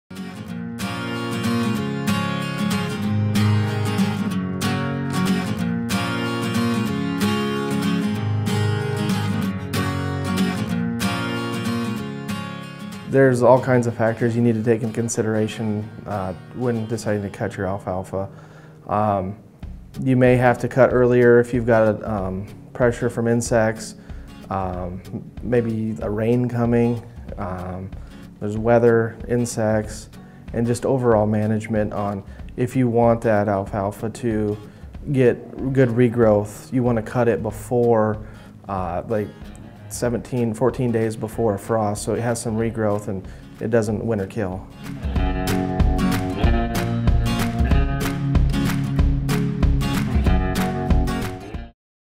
Gabe Fiscus, Goldfield, Iowa- Considerations for Cutting Alfalfa
Seedsman Gabe Fiscus discusses what factors farmers need to consider when deciding to cut alfalfa in this Ag Advice.